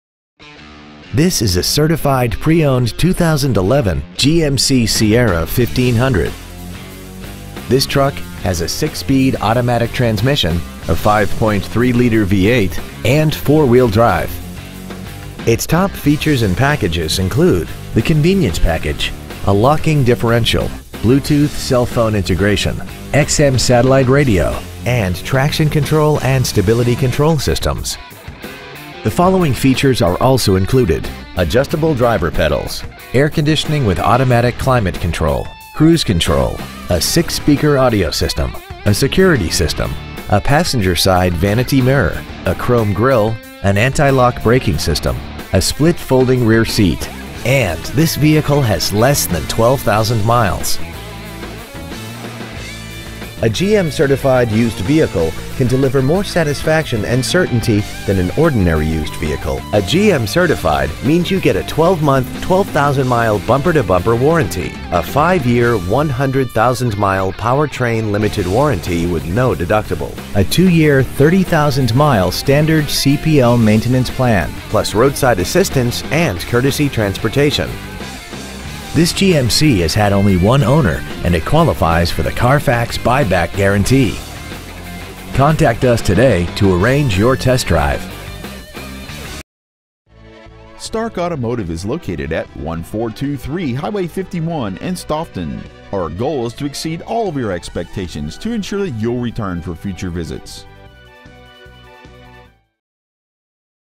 Used 2011 GMC Sierra 1500 Stoughton WI
Year : 2011
 Make : GMC
 Model : Sierra 1500
 Engine : vortec 5.3l v8 sfi vvt flexfuel
 Trans . : 6-speed automatic
 Exterior : gray green metallic
 Miles : 11,546
 Interior : ebony
 Stock : 3130075A